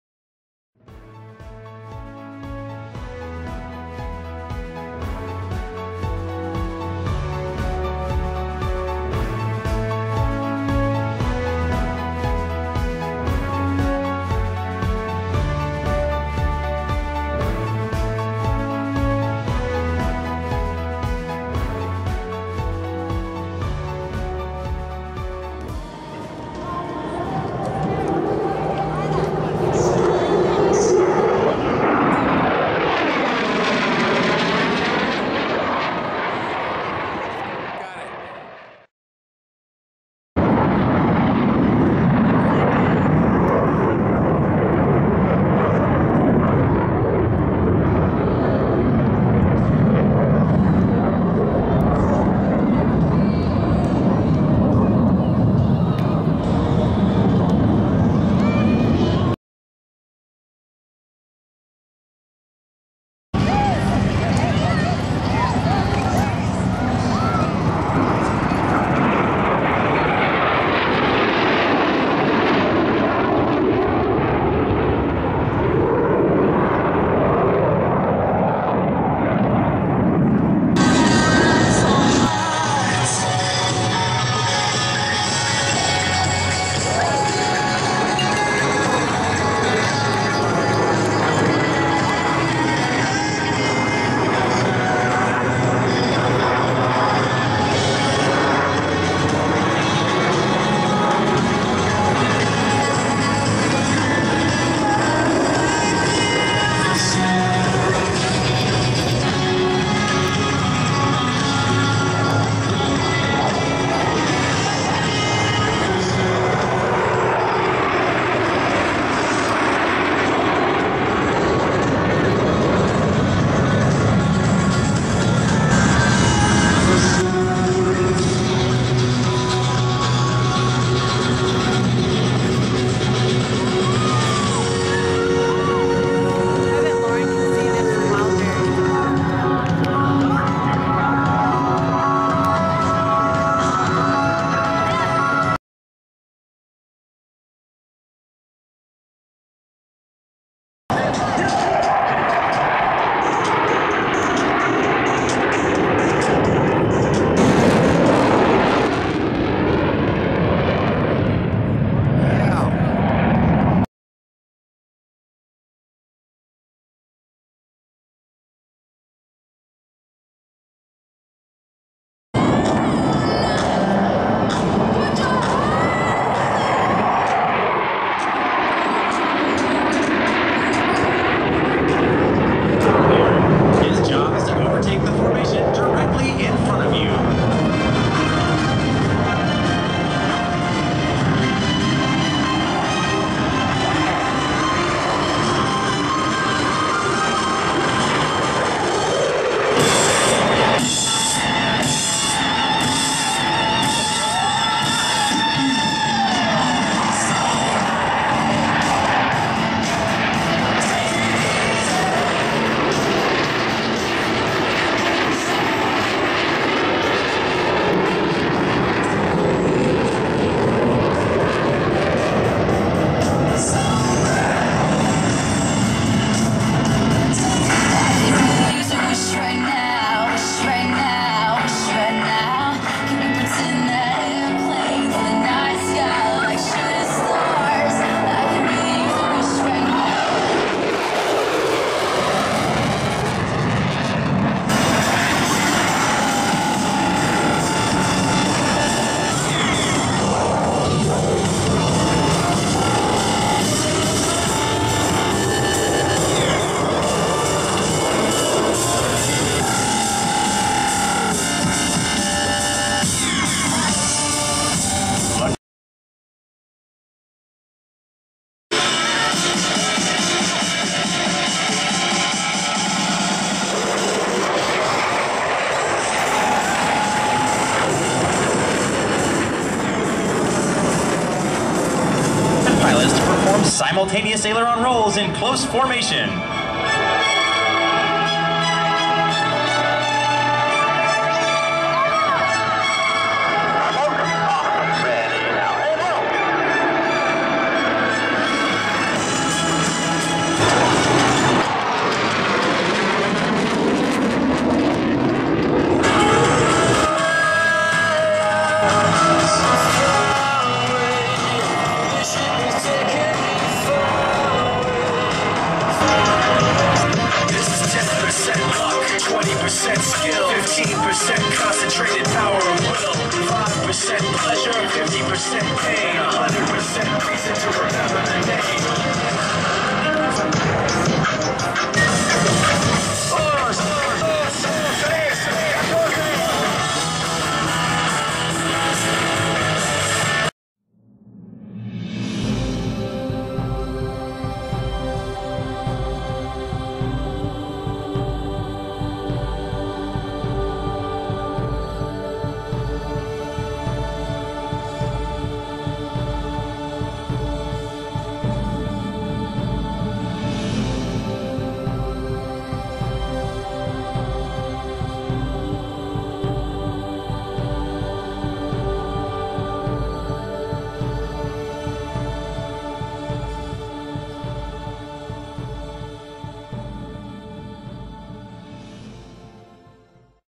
Arctic Thunder 2012
US AIR FORCE THUNDERBIRDS perform in Alaska